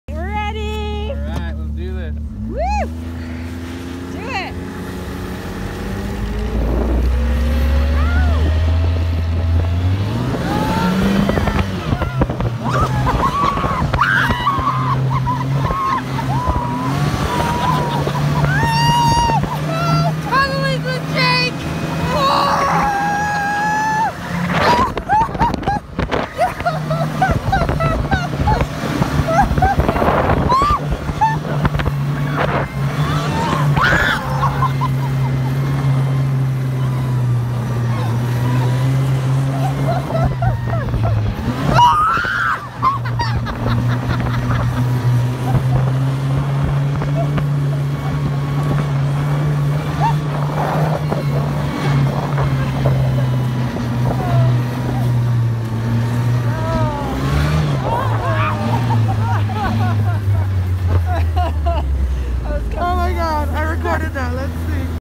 Barrow, Alaska by snow machine - 2016
Skidoo snow safari for three - Barrow, Alaska 2016

Adventure is always on the menu when you put three friends together, on a skidoo and in several feet of snow.  Let's not forget the sock monkey was invited too!